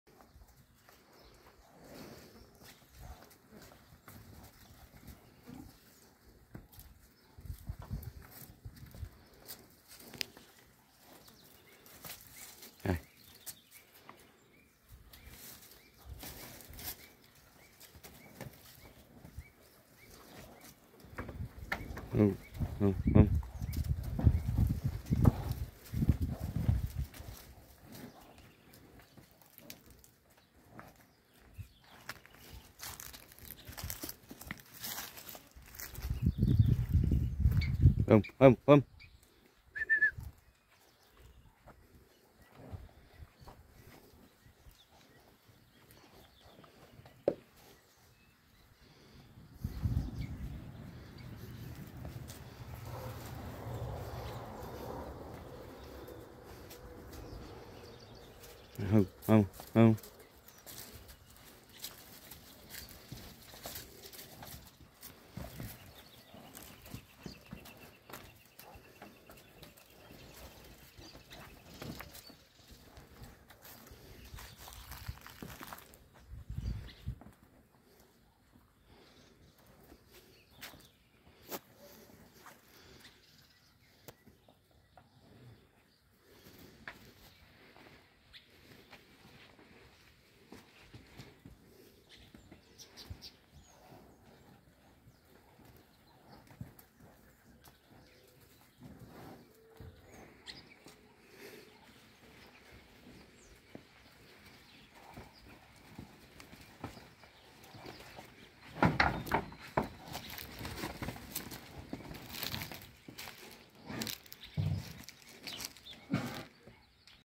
12 bezerros a R$ 1.900 cada em Pederneidas-SP/Tel: 14 99878-6814/João
Vende-se gado a 1.900 reais